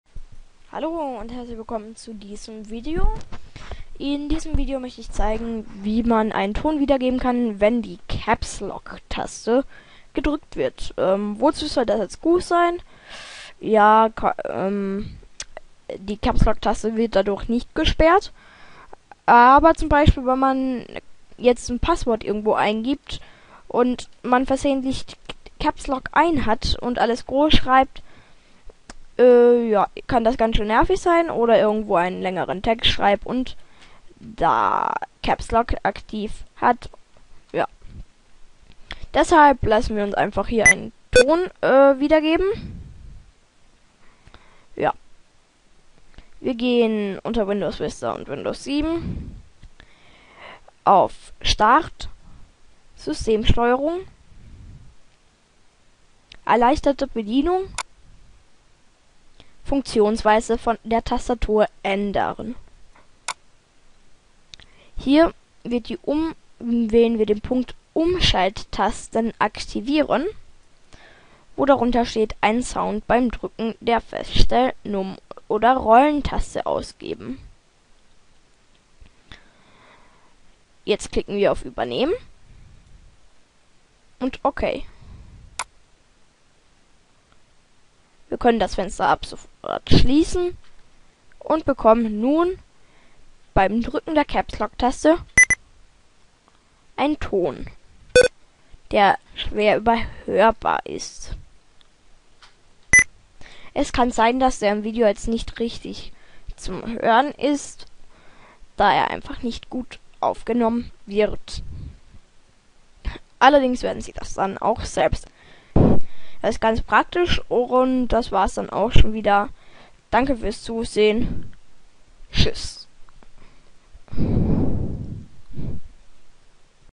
Ton beim Aktivieren von Capslock abspielen [ReUpload]
In diesem Video wird gezeigt, wie Sie beim Aktivieren von Capslock einen Ton abspielen können!